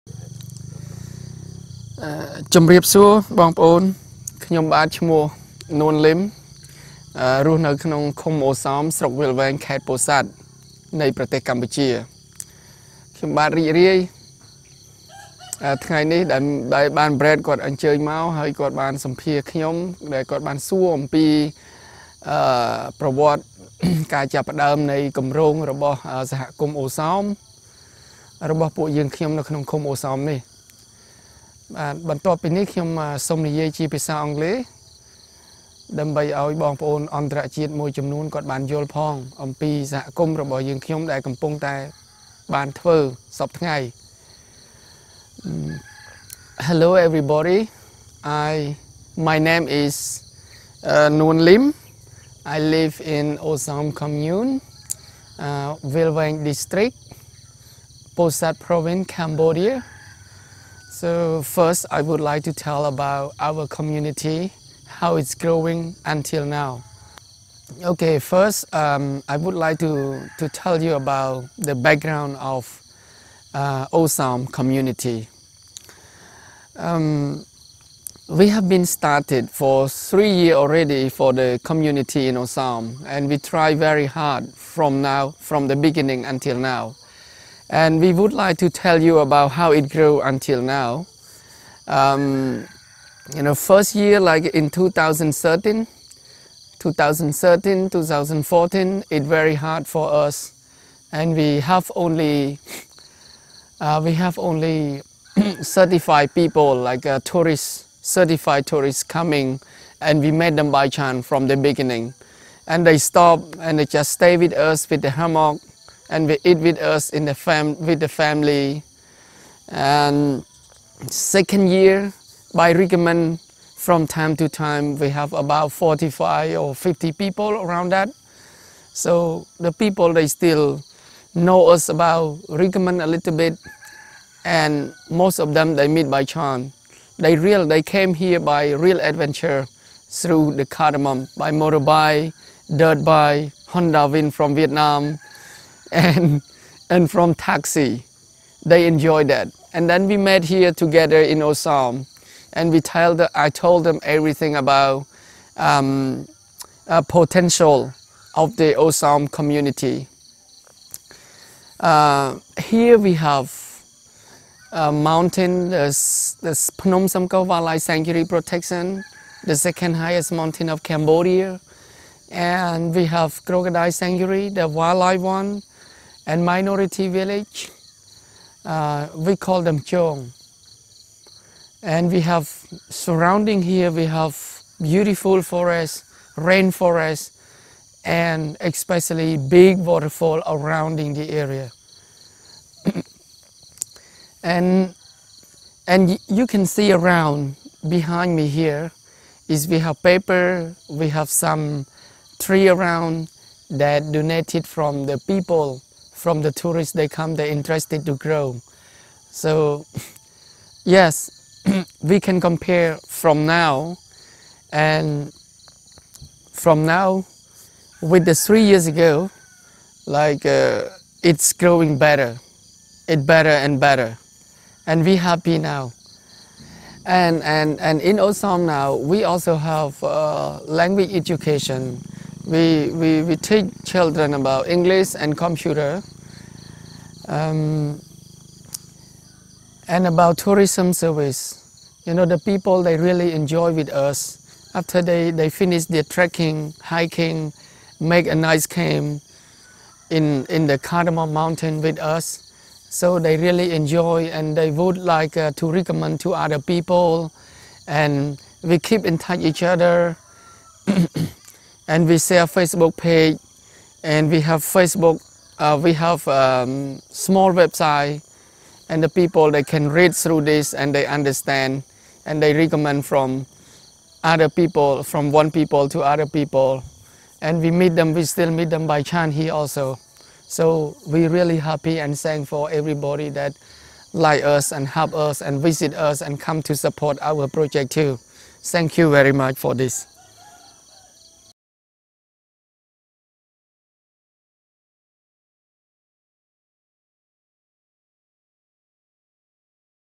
Osoam Community Center Interview // The full story from Lim
This video is part of a series of interviews conducted in the village of Osoam, Cambodia at the Osoam Cardamom Community Center. This village has a wonderful feeling of community continues to grown in popularity. The interview focused around understanding why people are travelling to Osoam and what they would tell others once they are ready to leave. Tourists often come from Koh Kong or Pursat by way of taxi through the jungle. Contact Lim for help arranging taxi or booking accommodations.

Created by Brad Ringstmeier 2018